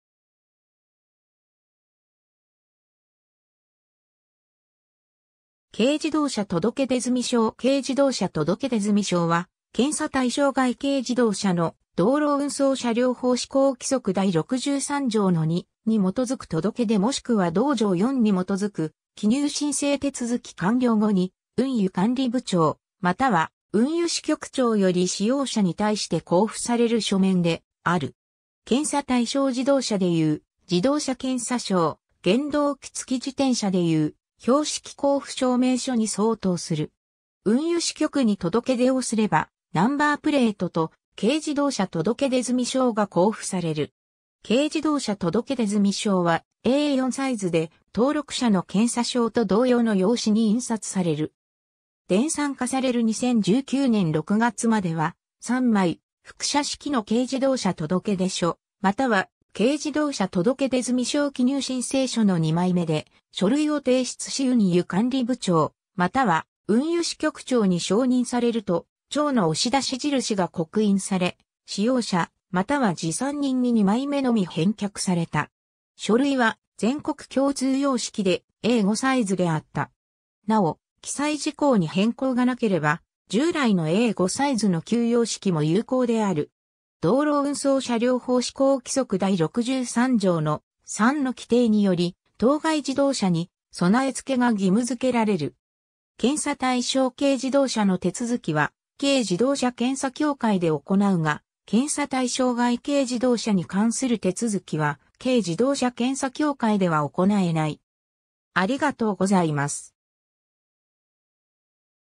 軽自動車届出済証
軽自動車届出済証 軽自動車届出済証（けいじどうしゃとどけでずみしょう）は、検査対象外軽自動車の道路運送車両法施行規則第63条の2に基づく届出もしくは同条4に基づく記入申請手続完了後に、運輸監理部長または運輸支局長より使用者に対して交付される書面である。
検査対象自動車でいう「自動車検査証」、原動機付自転車でいう「標識交付証明書」に相当する。
 運輸支局に届出をすればナンバープレートと「軽自動車届出済証」が交付される。
軽自動車届出済証はA4サイズで、登録車の検査証と同様の用紙に印刷される。
 電算化される2019年6月までは、3枚複写式の軽自動車届出書または軽自動車届出済証記入申請書の2枚目で、書類を提出し運輸監理部長または運輸支局長に承認されると、長の押出印が刻印され、使用者または持参人に2枚目のみ返却（交付）された。
書類は全国共通様式で、A5サイズであった。
なお記載事項に変更がなければ従来のA5サイズの旧様式も有効である。
 道路運送車両法施行規則第63条の3の規定により、当該自動車に備え付けが義務づけられる。
 検査対象軽自動車の手続きは軽自動車検査協会で行うが、検査対象外軽自動車に関する手続は軽自動車検査協会では行えない。
（2019年6月までは、地域によっては運輸支局長名義で軽自動車検査協会の職員が窓口対応して行っている場合もあったが、実際は運輸局職員が決裁していた。）